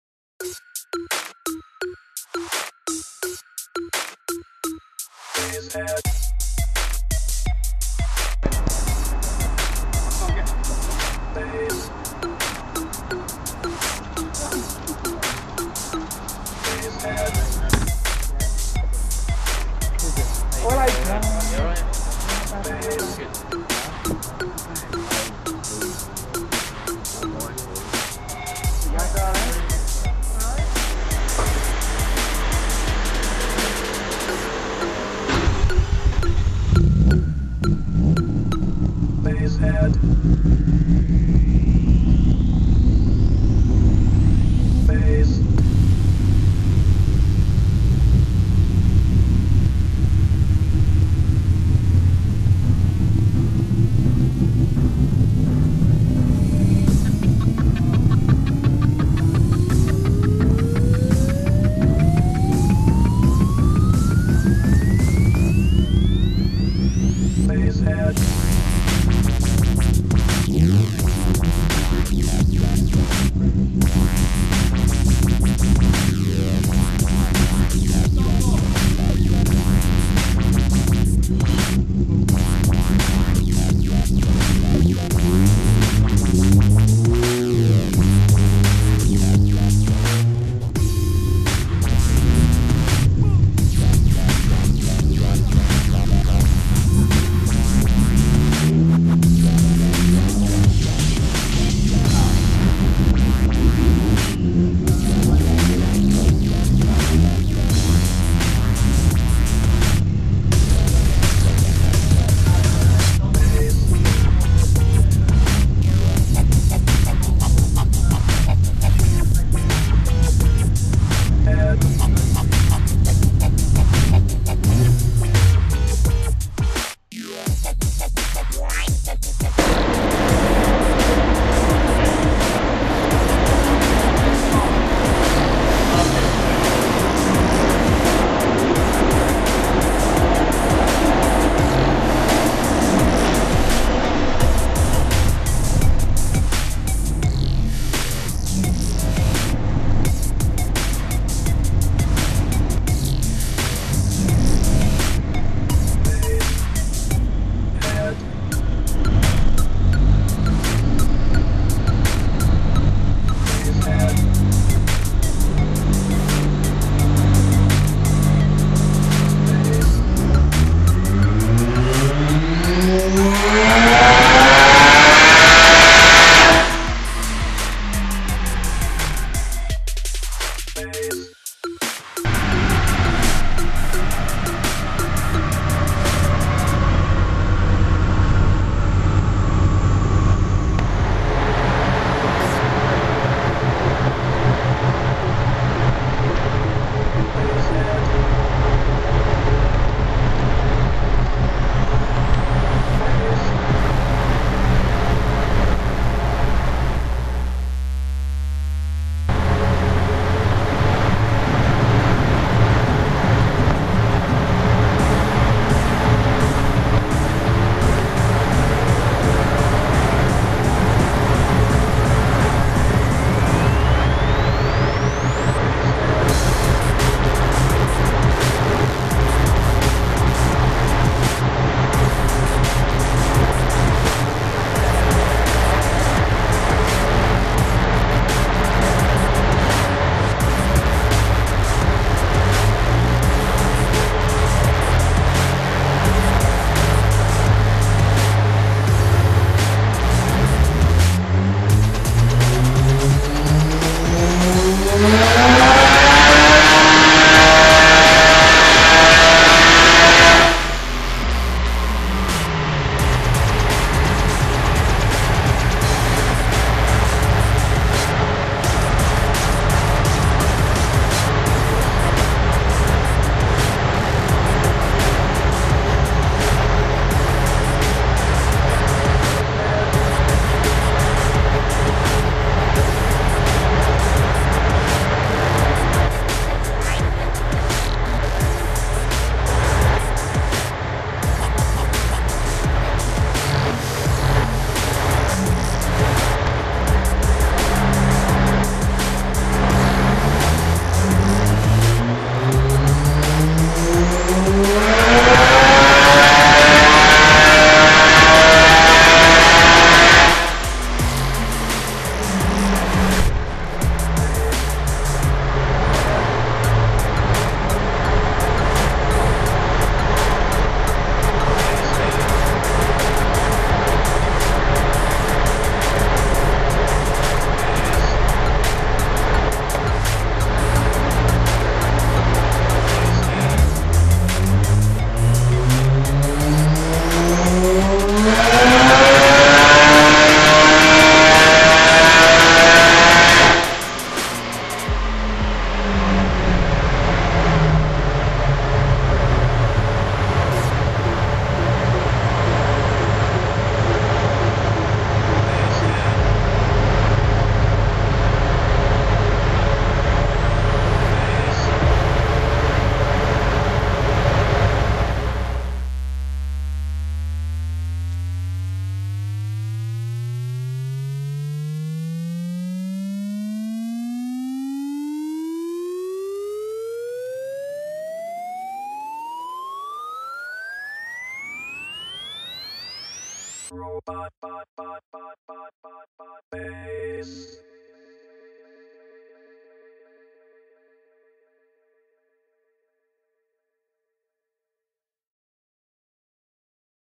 Oldone Racing Rx7 FD
Oldone Racing presents Rob Blackshaw Rx7 FD at a dyno mapping session.